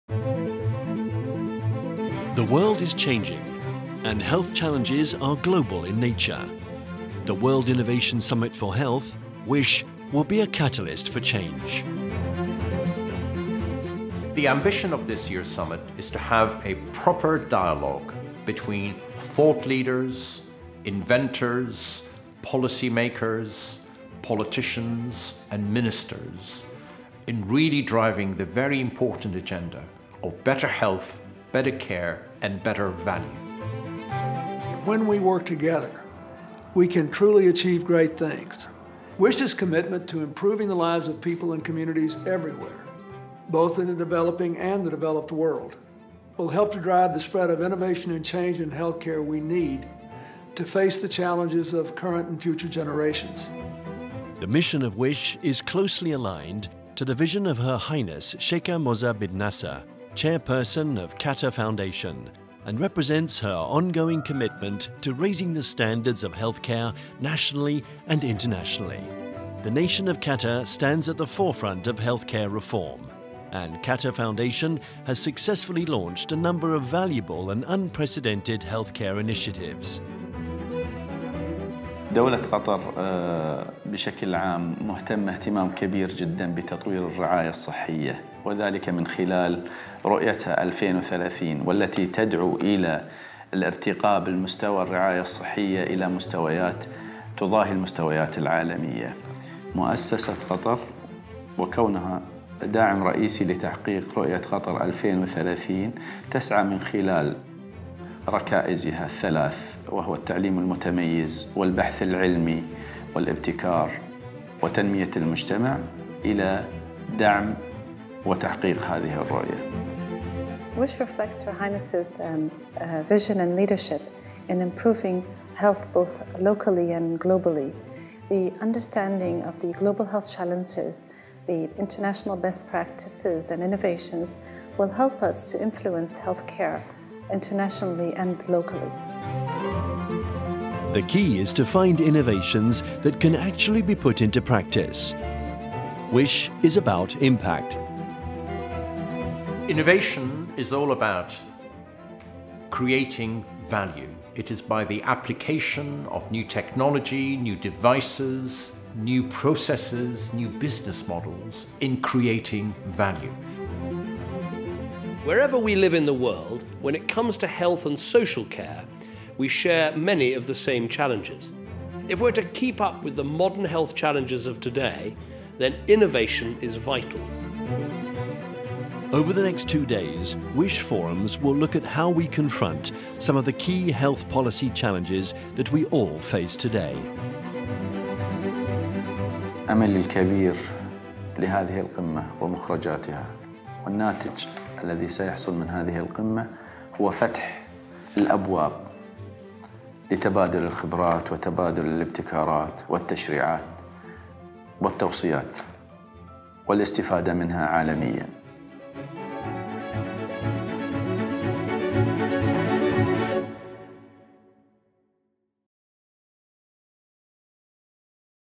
WISH 2013 Opening Film
Lord Darzi, Engineer Saad Al Muhannadi, Bill Clinton, David Cameron, Dr. Hanan Al Kuwari talk about how WISH will be a catalyst for change. Innovation is crucial to make a positive impact on healthcare reform.